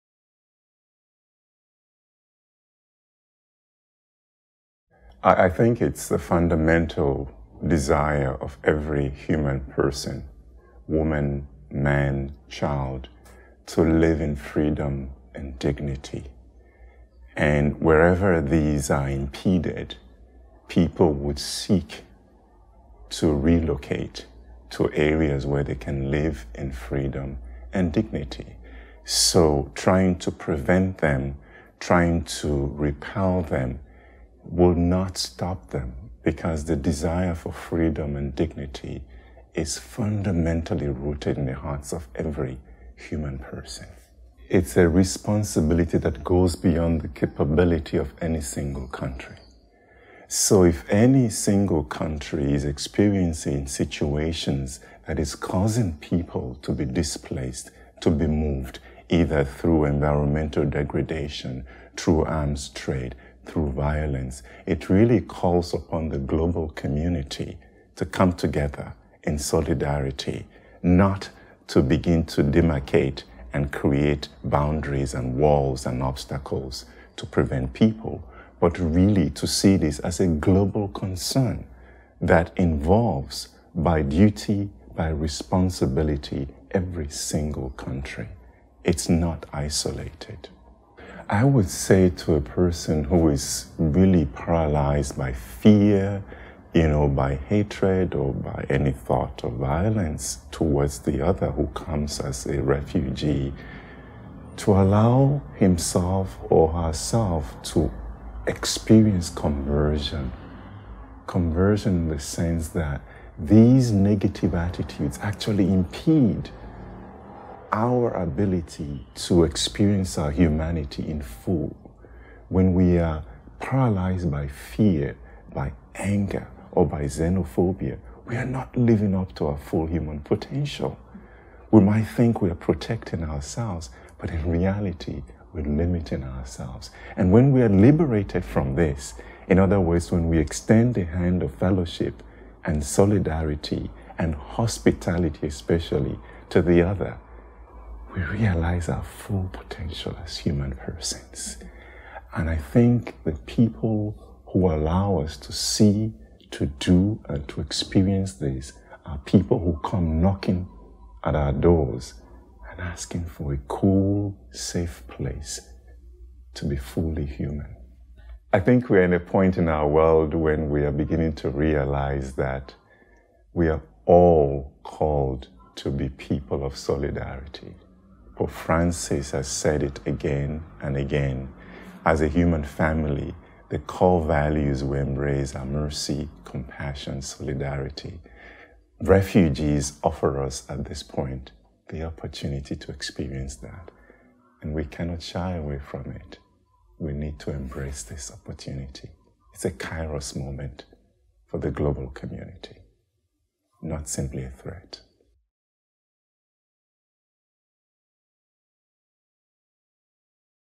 "it's a fundamental human desire to live in freedom"
P. Agbonkhianmeghe Orobator explains that, in a world with a divided stance on migrants and refugees, those who escape their country have an innate desire for two things: dignity and freedom. We need to view the influx of refugees and migrants less as a misfortune and more as an opportunity.